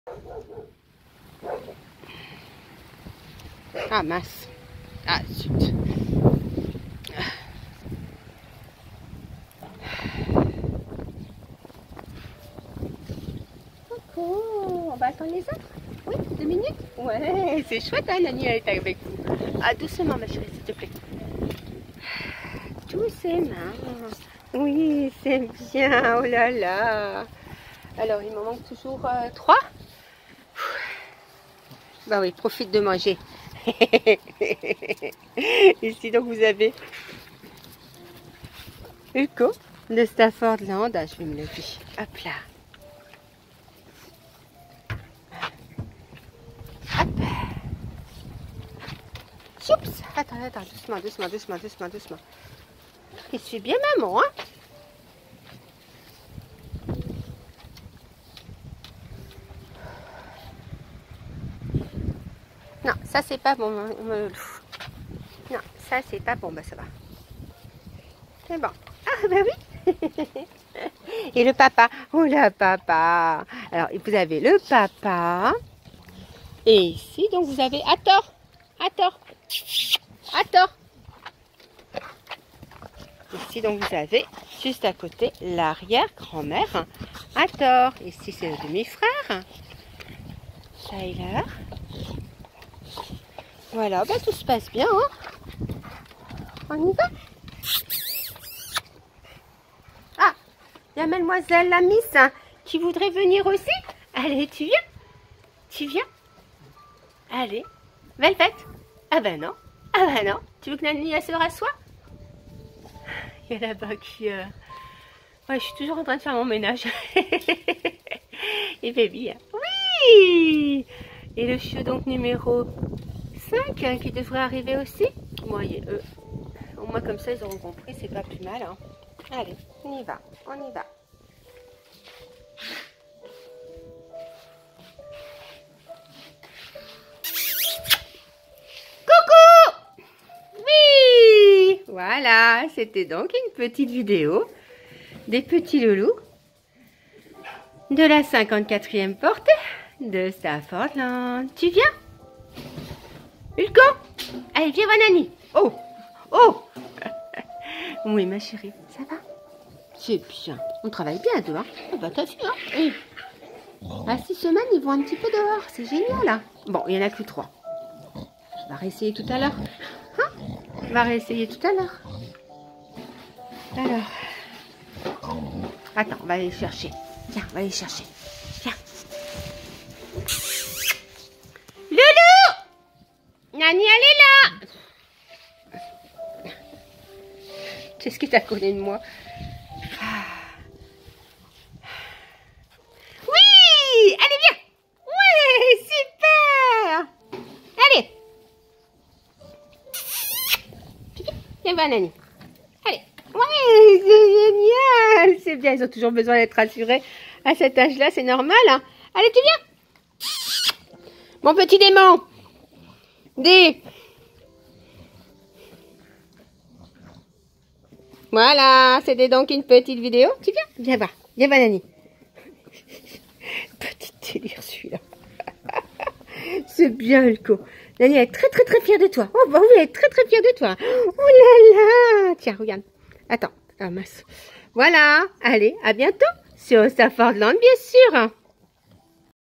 113ème vidéo Les Chiots Staffie de la 54ème Portée de STAFFORDLAND29
Les Chiots de la 54ème Portée n'ont que 6 semaines de naissance sur cette vidéo et les parents de cette portée sont RIDER RED DE LA CRIQUE DU FLOJULE et de RUBIS de STAFFORDLAND

Ceux sont des chiot, chien, chiens de race Staffordshire Bull Terrier dit Staffy, Staffi, Staffie, Stafford